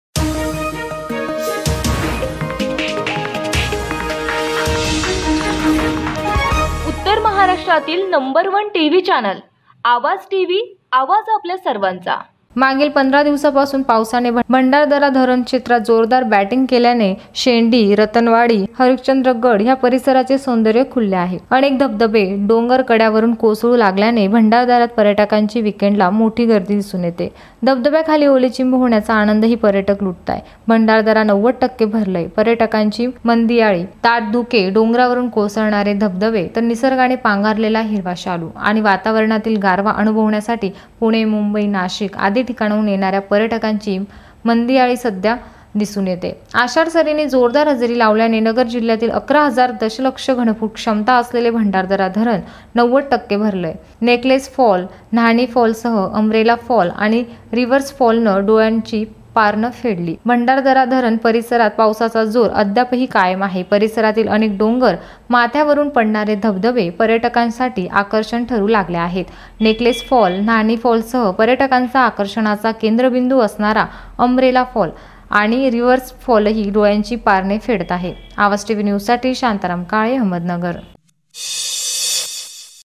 Aawaj Tv
अहमदनगर |  भंडारदरा परिसरात पावसाची जोरदार बॅटिंग, शेंडी, रतनवाडी, हरिश्चंद्र गड पाहण्यासाठी पर्यटकांची गर्दी